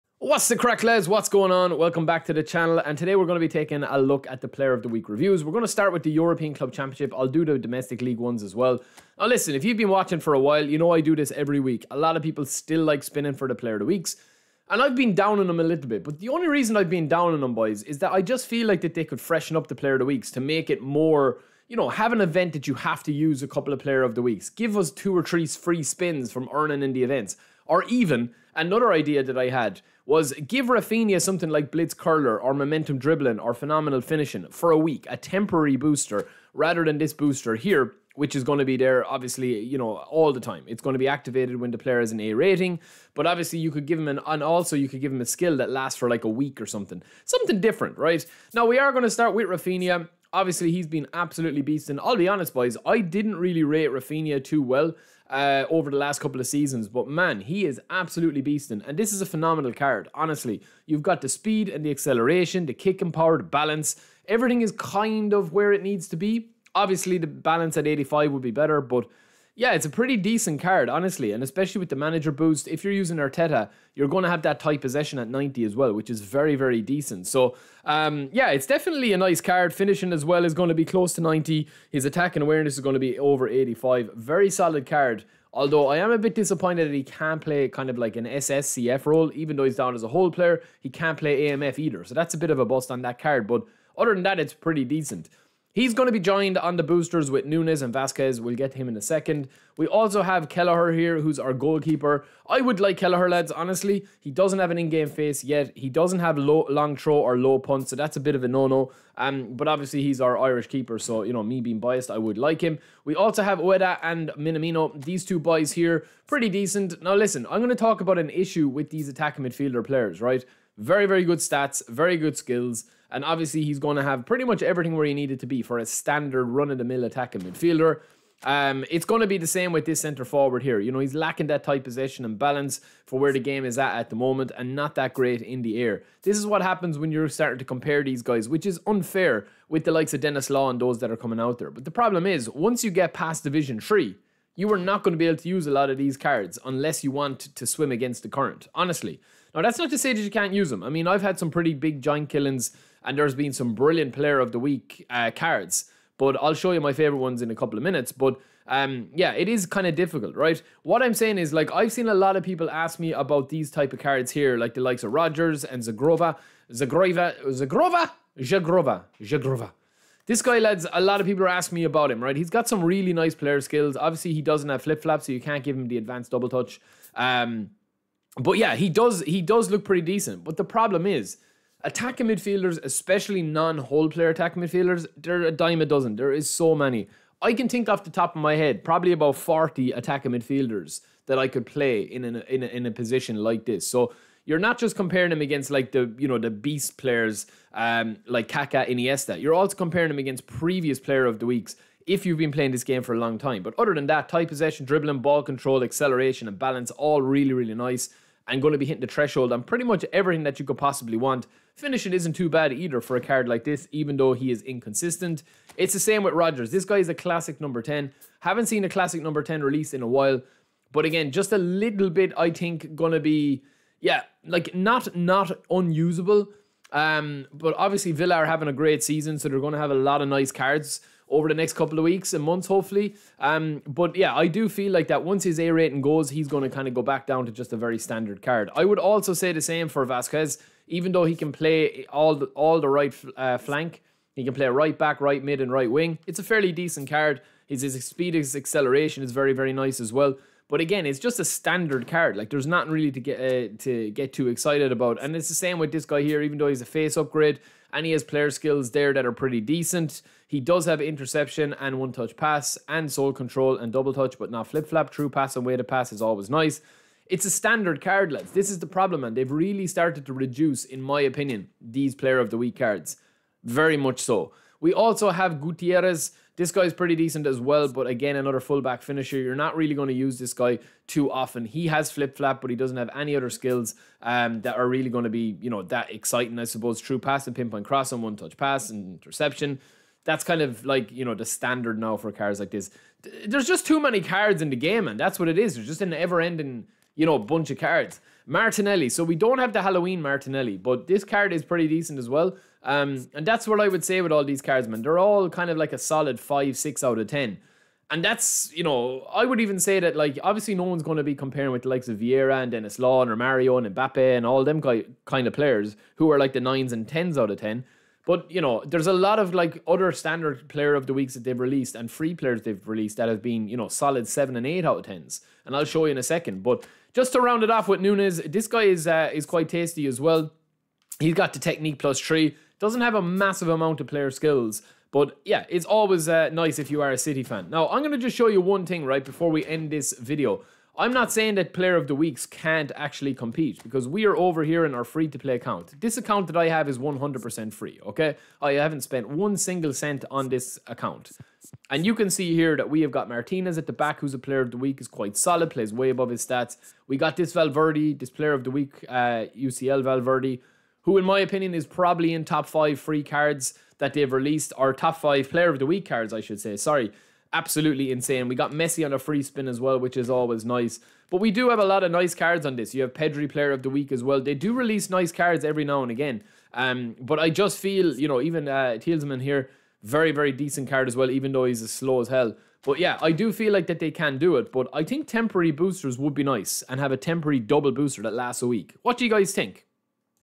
POTW REVIEW ft. RAPHINHA, MARTINELLI & NUNES
Champions League POTW is back and features Raphinha, Martinelli, Gutierrez, Reijnders, Vasquez, Minamino, Kelleher, Zhegrova, Rogers and Ueda.